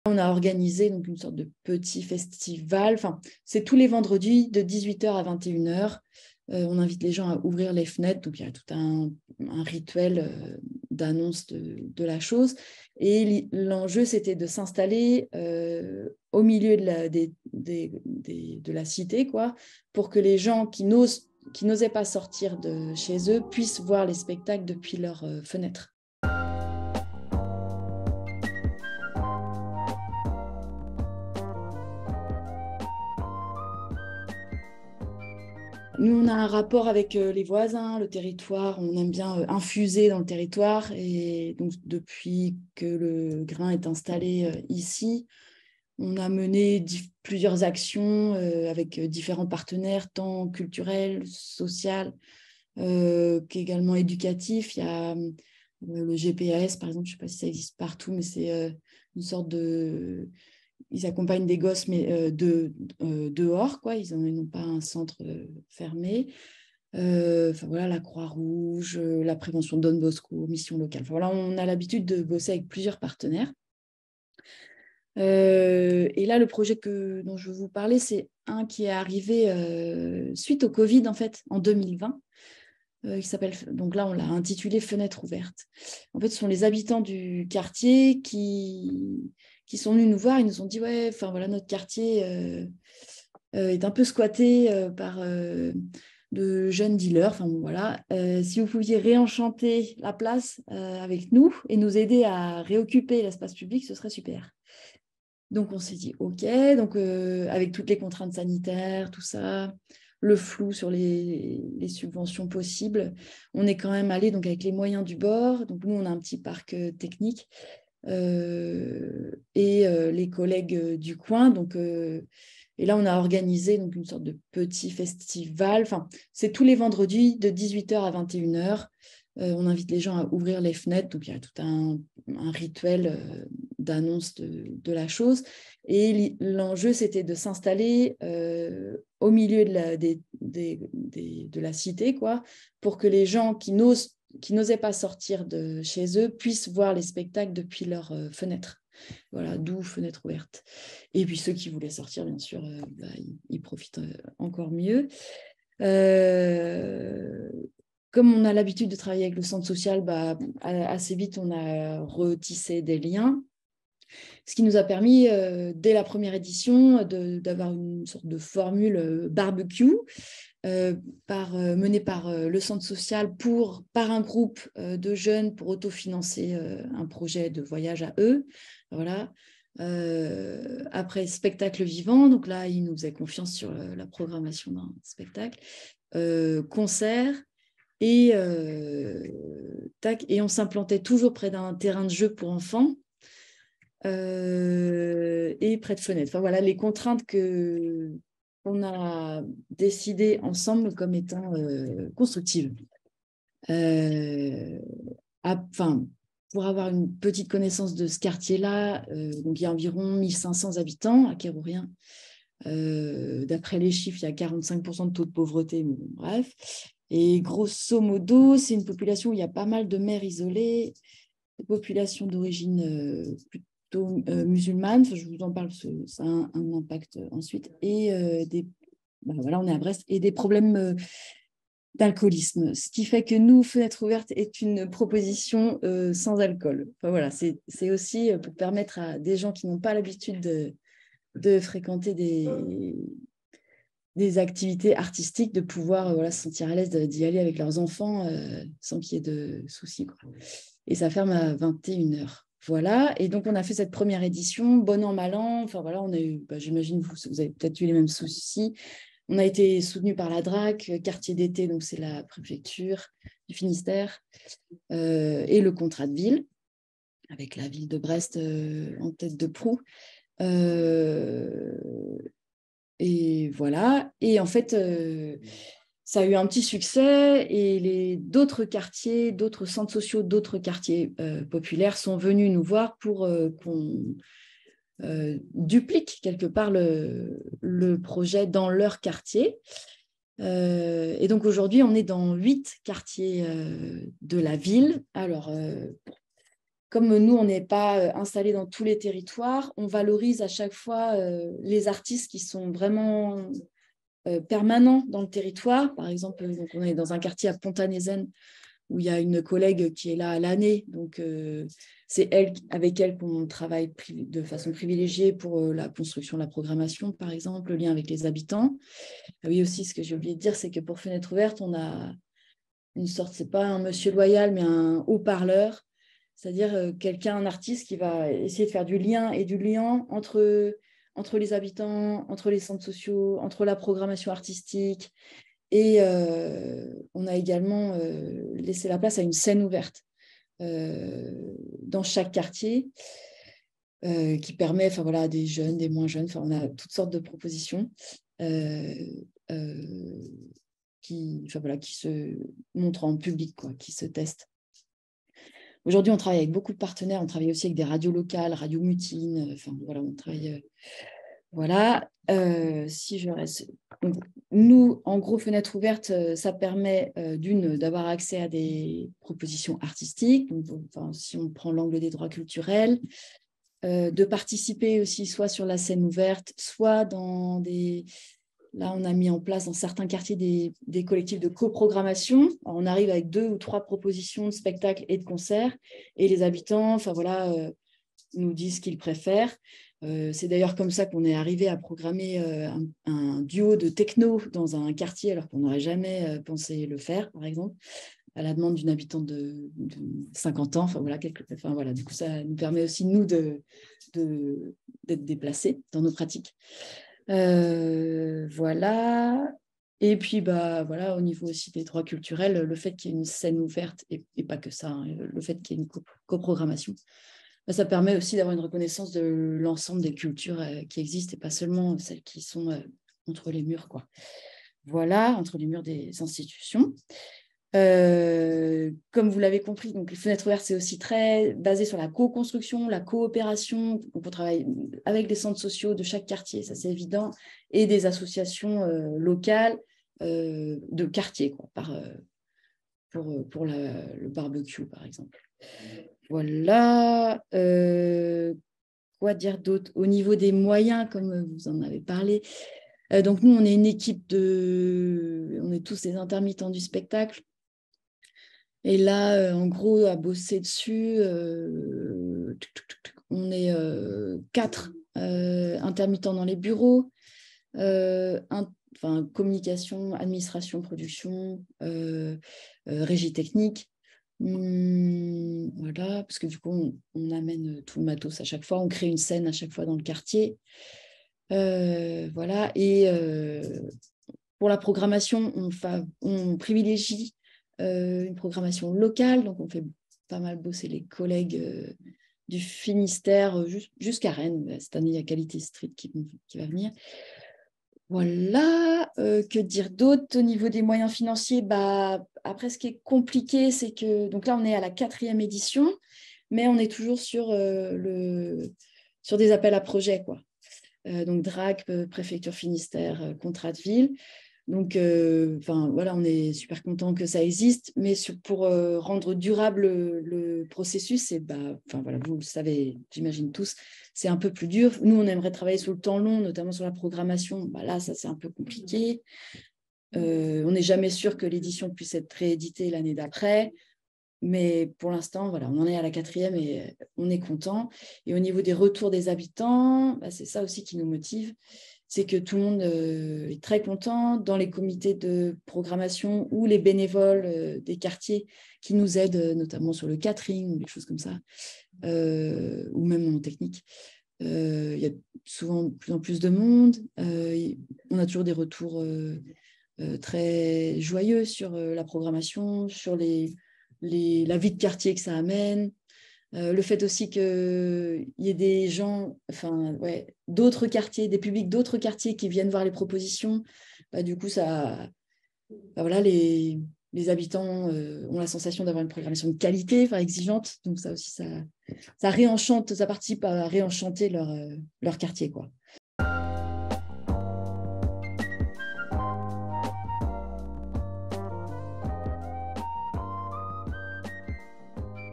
Pitch Le Théâtre du Grain
Présentation du Théâtre du Grain et de ses actions estivales